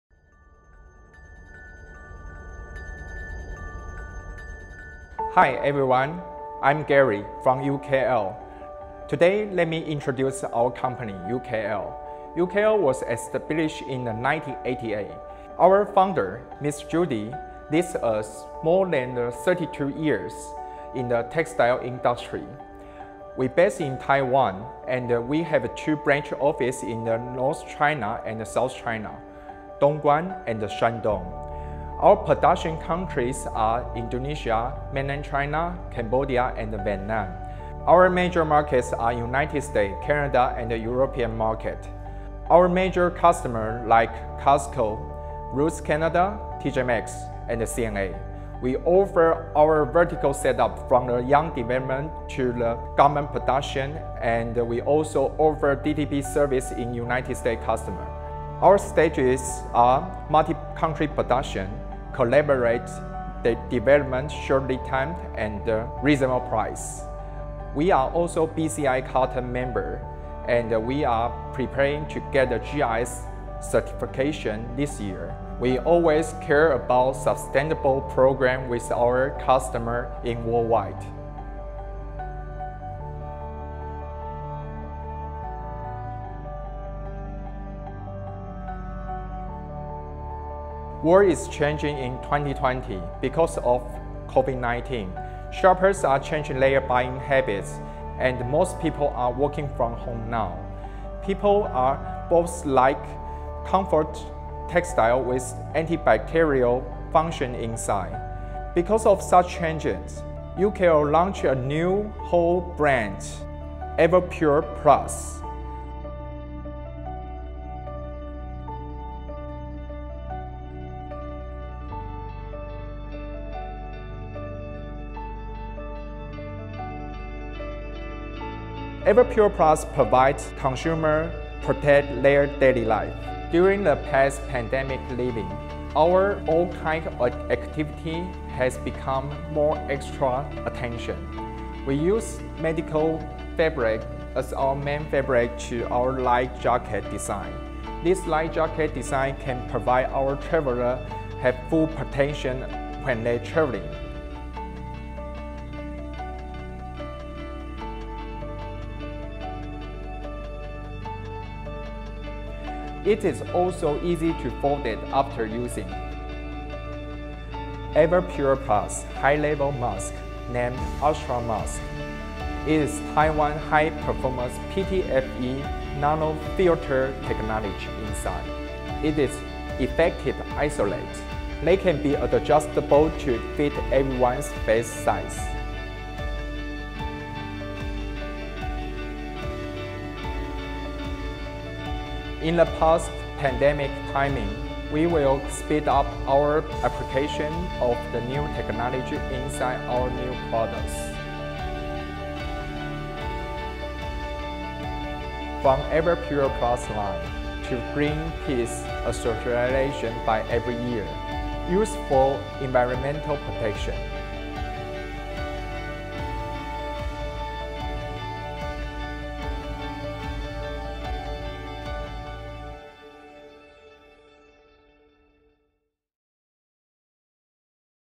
EVOPURE+ and ASTRO Mask
Presenter: Mr. Gary Hsieh, Executive Vice President of UKL Enterprise Co., Ltd.

EVOPURE+ is a new launch brand during COVID-19 outbreak by UKL. We applied Nano-Zinc high technology in our product designed. All EVOPURE+ products are made in Taiwan high quality production line.

ASTRO MASK (High Level Mask):
This mask is high level mask in EVOPURE+ collection. HIGH PERFORMANCE ANTIBACTERIAL FABRIC MASK WITH PTFE NANO FILTER to block PM2.5 effectively
With 3D tailored design, stylish and go with your face, exclusive and patented NANO-ZINC technology, long-lasting antibacterial and deodorizing, premium surface fabrics for comfortable, breathable, and non-muggy wearing.
It is adjustable earmuffs design, easy to adjust, and comfortable to reduce pressure.
The most important point is robust and durable for more than 50 repeated washing.

PROTECTIVE MASK
Simple product, Stylish looks, 3D tailored, increased breathing space, breathable and comfortable, made with care to cover every inch of your skin gently, repeated washing without deformation, balance environmental protection and durability, long lasting bacteria prevention featuring exclusive patented NANO-ZINC technology not only blocks dirt and suspended particles in the air but also makes you look stylish!